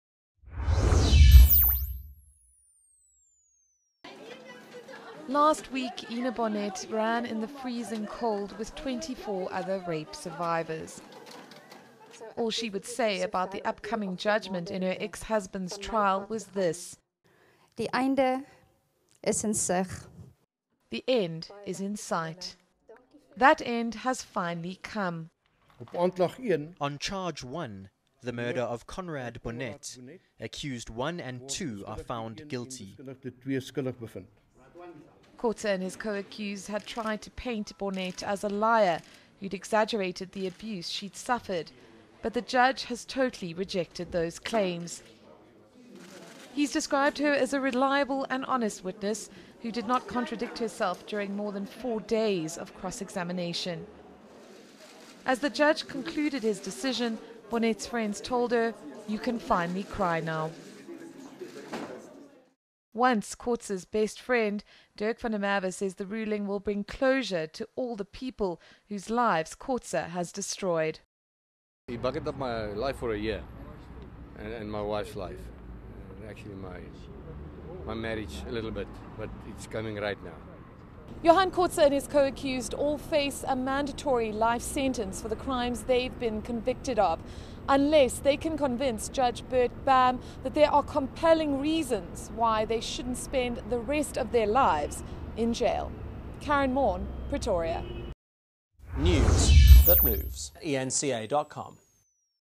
Kotze found guilty
Johannesburg, July 15 -- The so-called Modimolle Monster is guilty.  Johan Kotze has been convicted on four charges relating to the assault of his ex-wife Ina Bonette and the murder of her son Conrad.  His co-accused Andries Sithole has also been found guilty of murder.